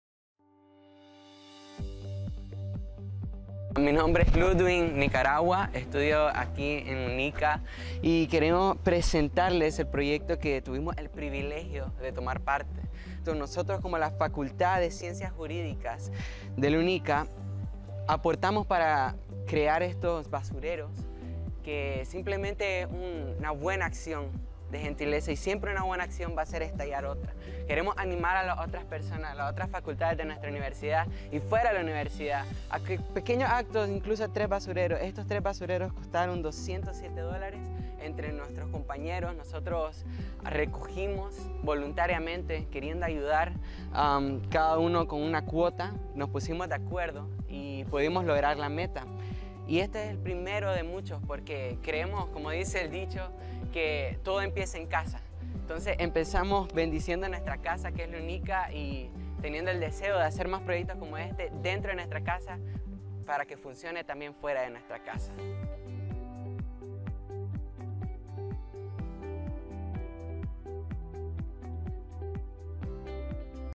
Donación depósitos de basuras
Estudiantes de la Facultad de Ciencias Jurídicas y Sociales donan depósitos de basura.